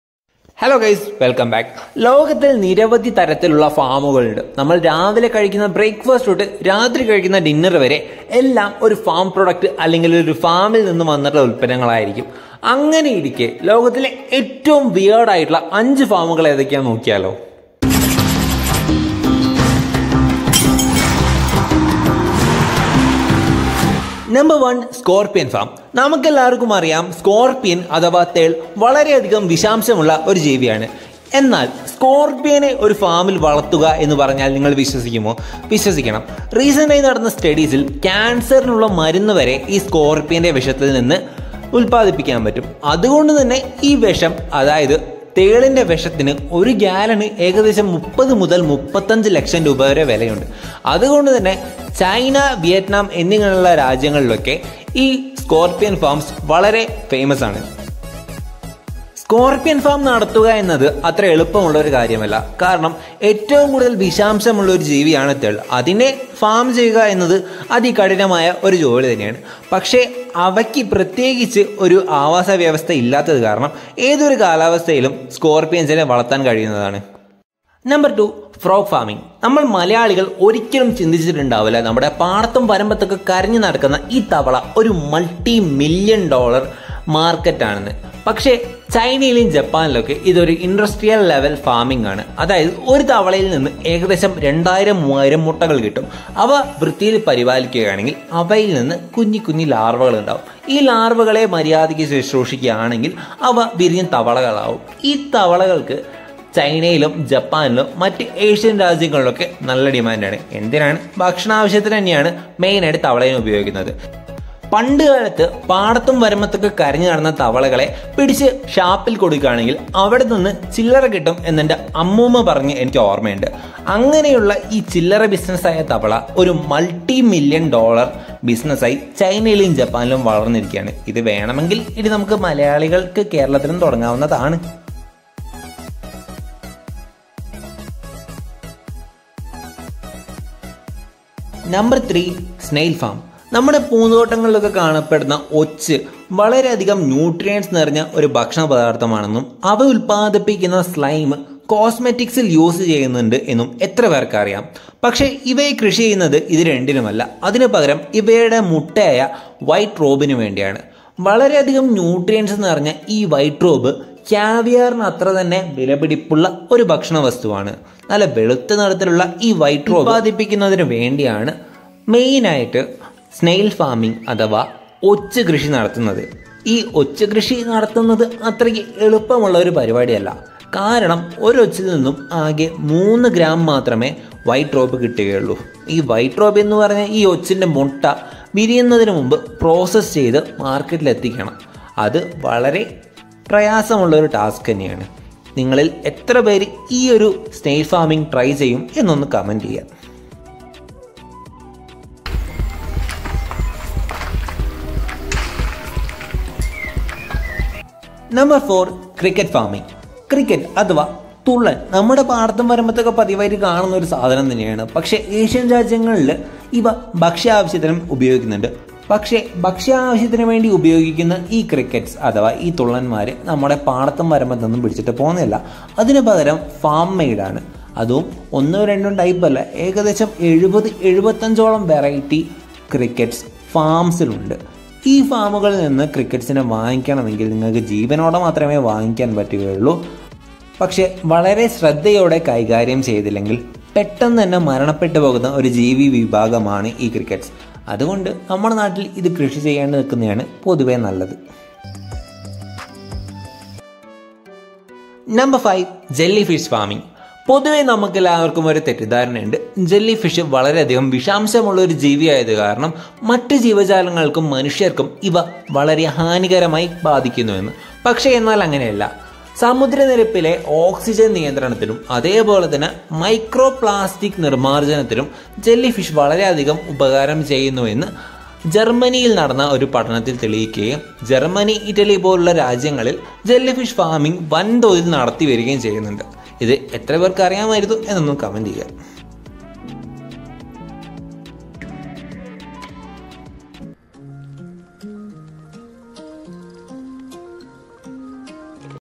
തേൾ മുതൽ തവള വരെ 😳| top 5 weird farms on earth
let's look at some interesting farms around the world which you might not even have heard of, and explore why the exotic animals are being farmed wideworld facts cricket wayanadanwalker top 5 farms, top 5 Malayalam

00:00 Intro
00:24 Scorpion Farm
01:14 Frog farming
02:23 Snail farm
03:29 cricket farm
04:15 Jelly fish farm